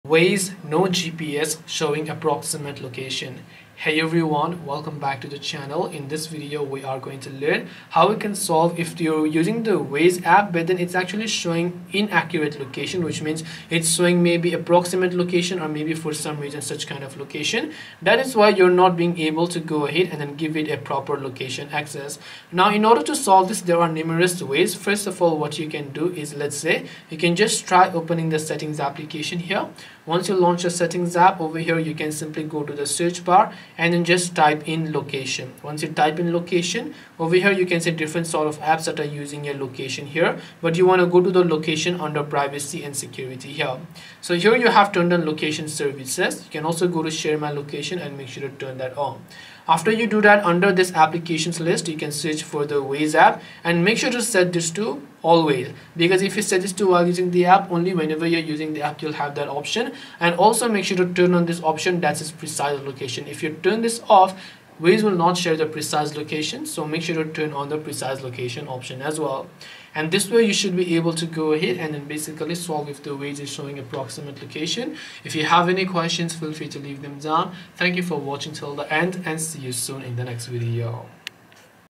Waze No GPS Showing Approximate Location
In this tutorial video, I will simply show you how you can solve if the Waze app is showing an approximate location.

~Chapters:
0:00 Introduction
0:15 Fix Waze Location
1:10 Conclusion